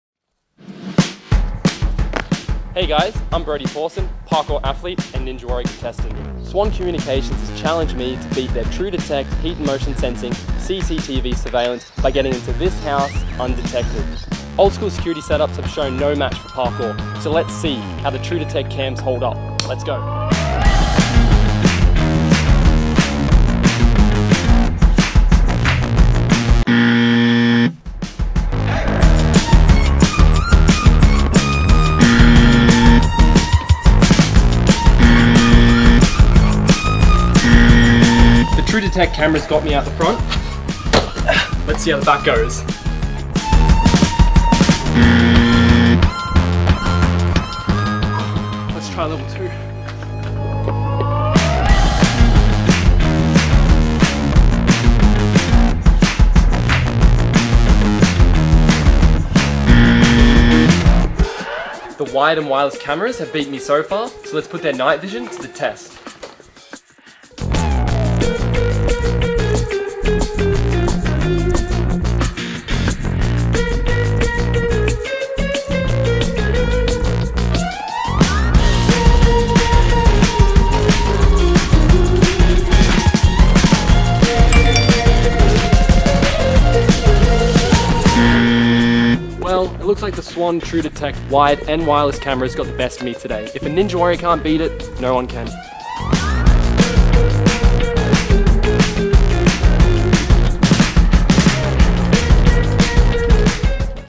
Ninja Warrior vs Swann Security Cameras. Who Wins? (Full Version)
We challenged Ninja Warrior contestant and parkour athlete, Brodie Pawson, to beat Swann's True Detect security cameras. Is he fast enough to avoid triggering the heat and motion sensors? We tested wired and wireless security cameras, day and night to find out!

Want to buy your own Swann True Detect product? Available on Swann.com